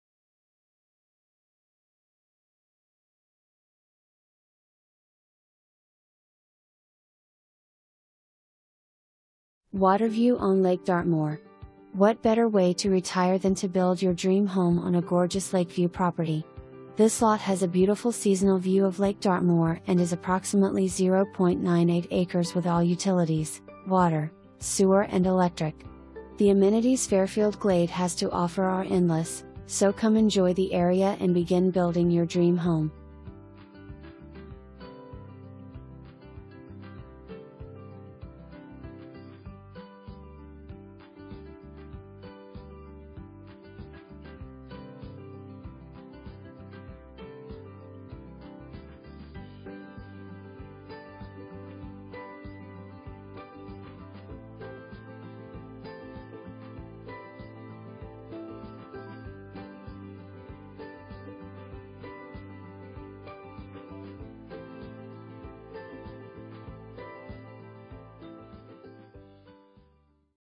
44 brummel Lane
WATERVIEW ON LAKE DARTMOOR! What better way to retire than to build your dream home on a gorgeous lakeview property. This lot has a beautiful seasonal view of Lake Dartmoor and is approximately 0.98 acres with all utilities; water, sewer and electric. The amenities Fairfield Glade has to offer are endless, so come enjoy the area and begin building your dream home.

Victoria Eberhart
Coldwell Banker Wallace